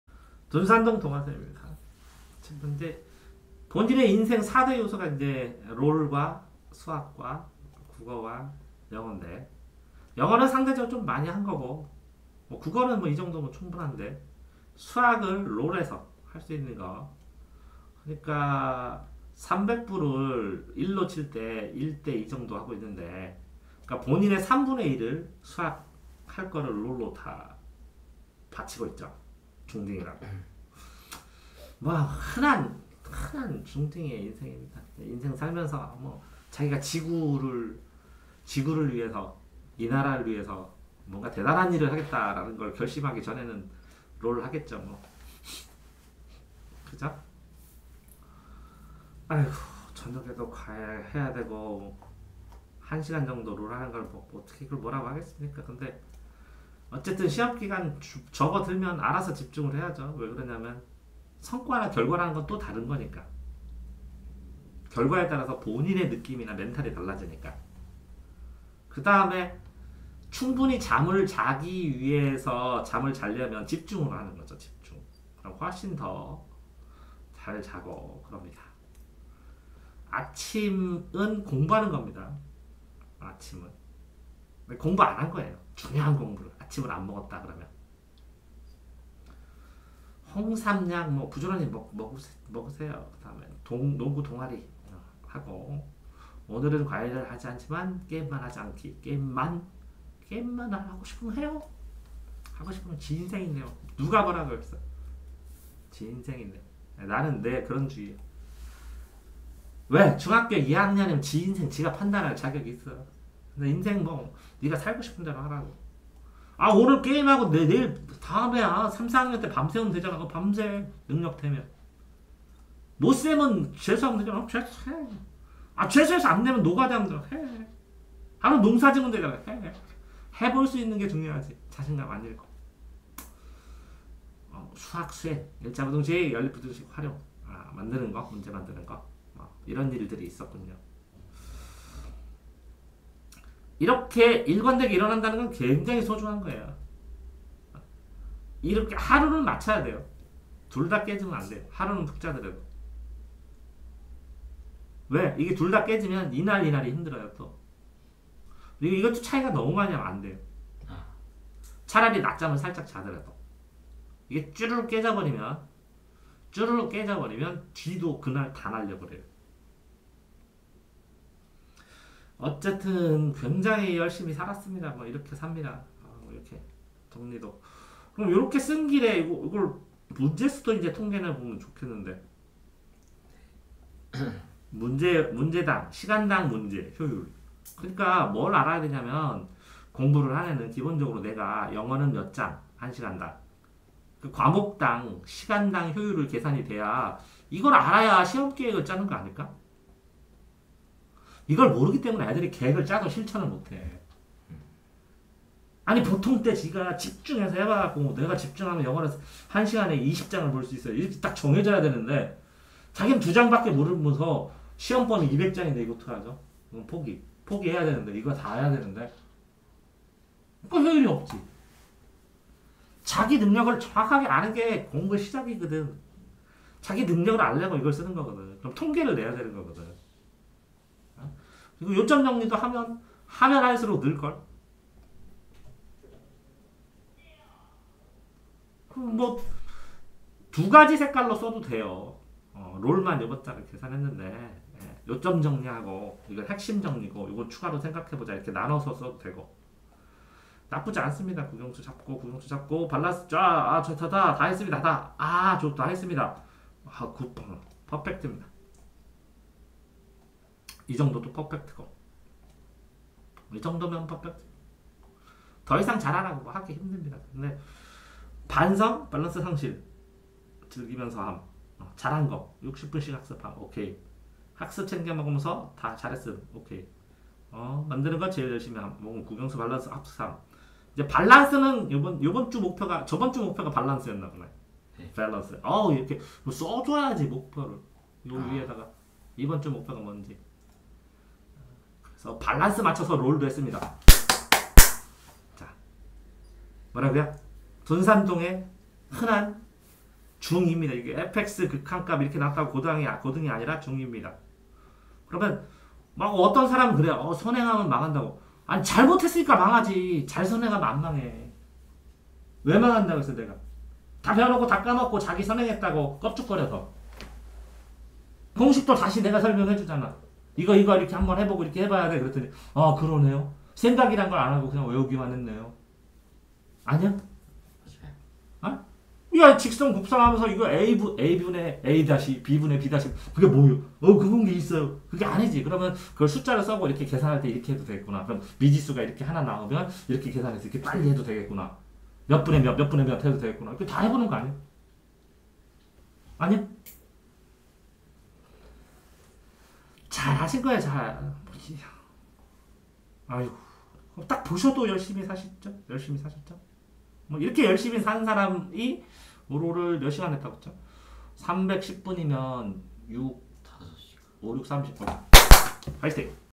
5725 [ 상담 ] 대전 둔산동 흔한 성실한 중2 일주일 생활
상위권 중2 미적분 선행 중